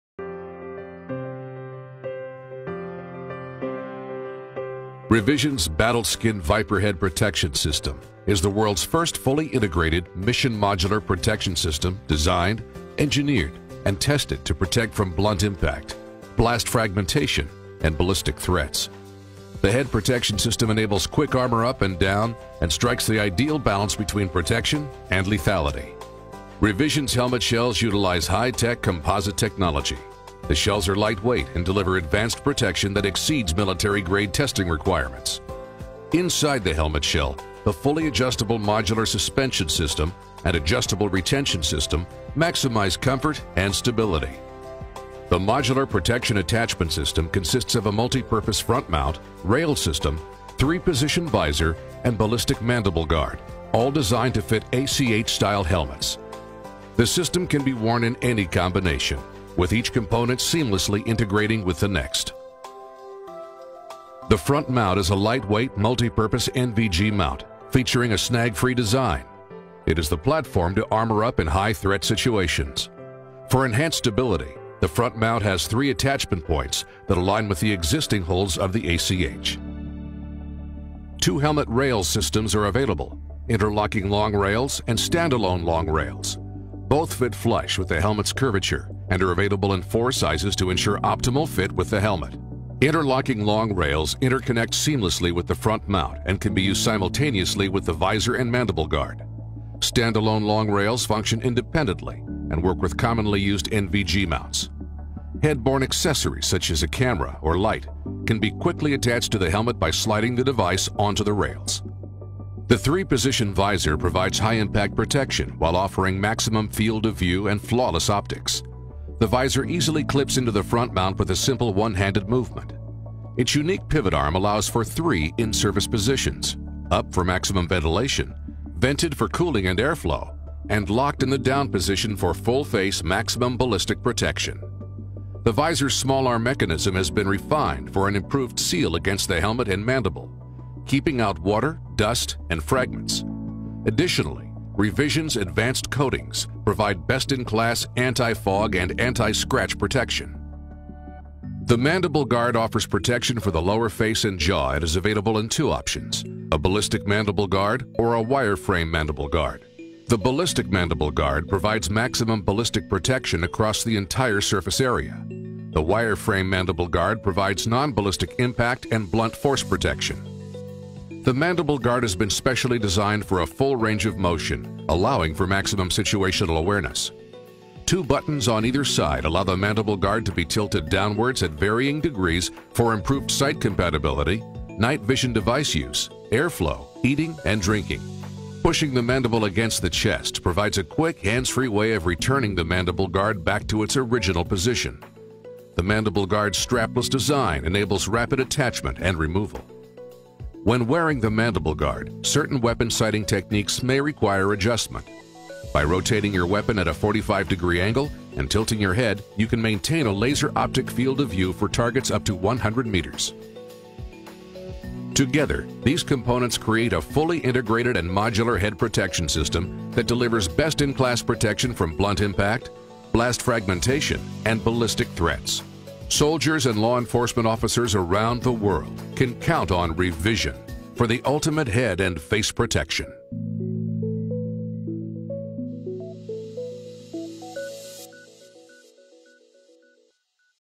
Revision’s Batlskin Viper Head Protection System
Revision’s Batlskin Viper Head Protection System is the world’s first fully integrated, mission modular head protection system designed, engineered, and tested to protect from blunt impact, blast fragmentation, and ballistic threats.